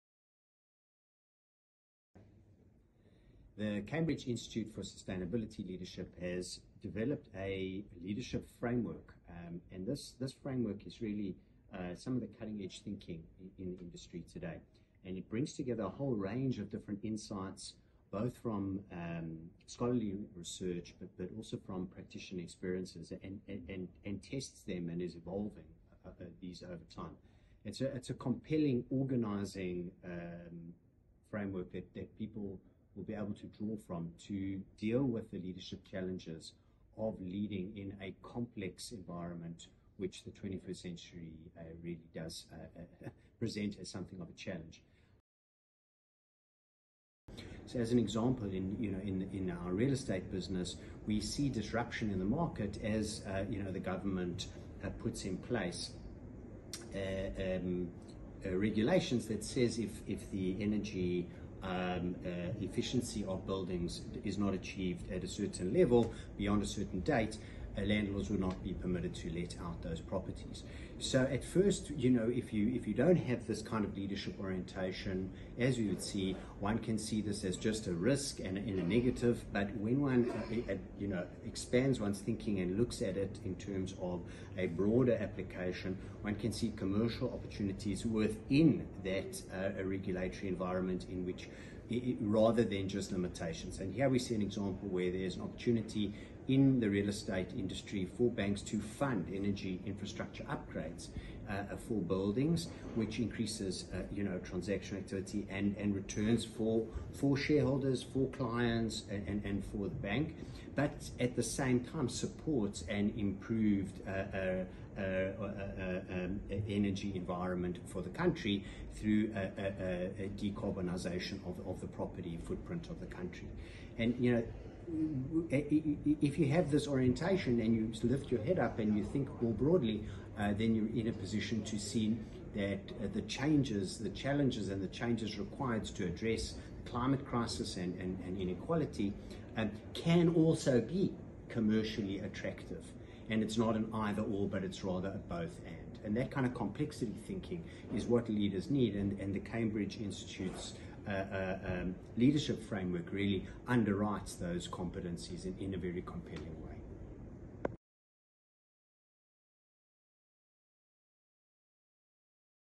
Marc Kahn, Investec’s Chief Strategy Officer, discusses the CISL's Leadership Framework
The time to reshape leadership is now. We need to broaden our vision to tackle the climate crisis and inequality, while also recognising the commercial potential. It’s not an either/or, but a both/and. This is the kind of complex thinking leaders need today. Cambridge Institute for Sustainability Leadership’s 'Leadership for a Sustainable Future' framework equips leaders with these critical competencies.

Marc Kahn, Investec’s Chief Strategy & Sustainability Officer, discusses this powerful framework. By drawing on real challenges faced at Investec and examples of stakeholder capitalism, Marc demonstrates how leaders can use this framework to go beyond transactional activity and achieve outcomes that benefit both people and the planet.